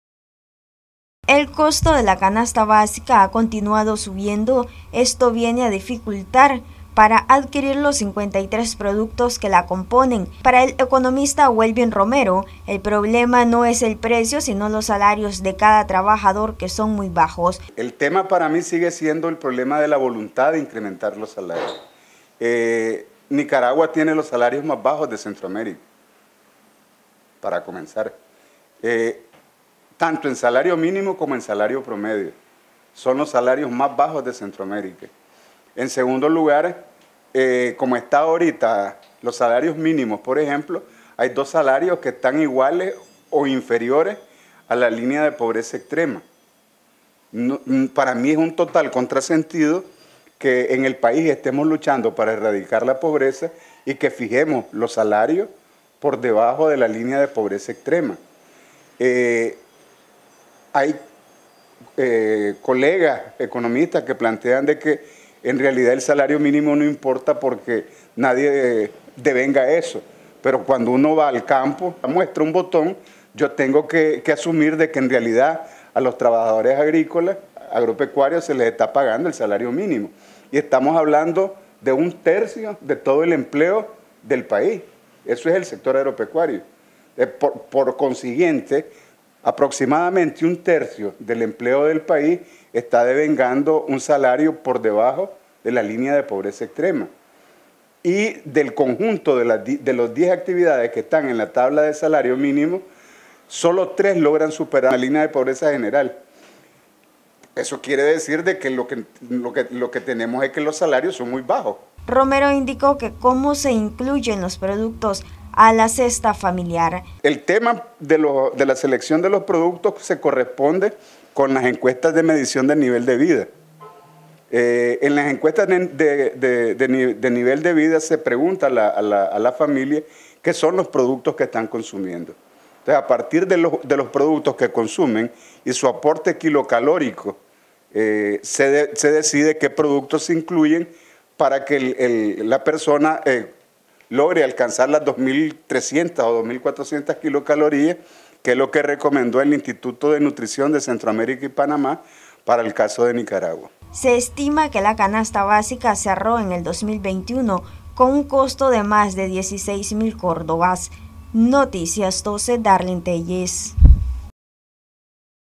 Bajos salarios impiden la accesibilidad a todos los productos de la canasta básica
Noticias12Nicaragua Economista Welbin Romero dio a conocer que la dificultad de poder adquirir todos los productos de la canasta básica se debe a los bajos salarios.